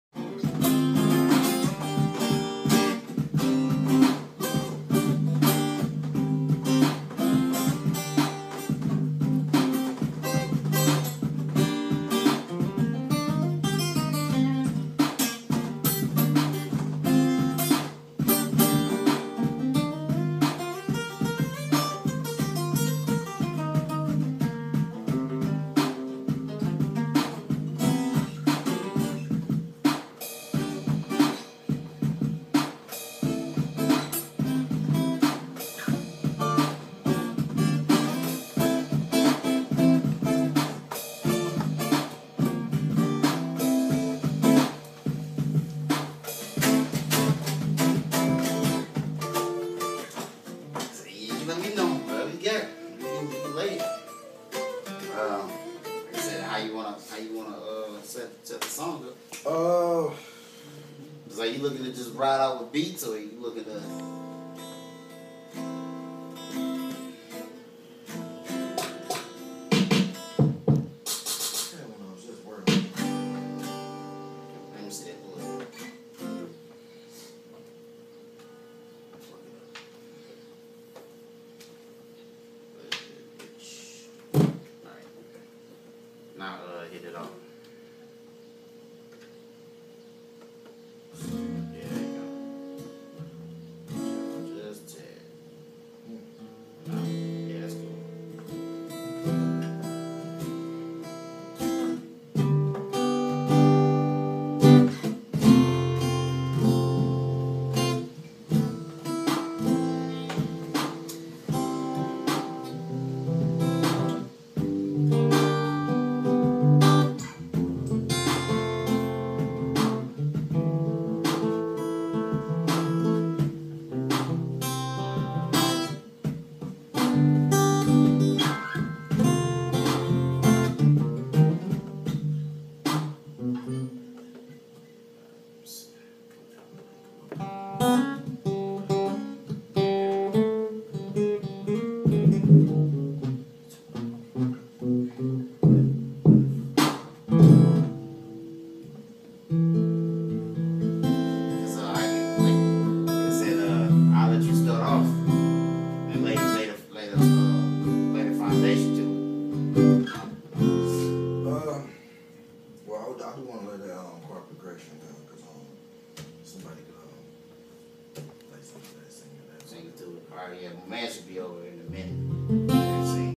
Me and AJ Doing Work In The Lab
Just a little something that me and AJ did in the lab yesterday. Just getting a idea on some projects that Finna come up.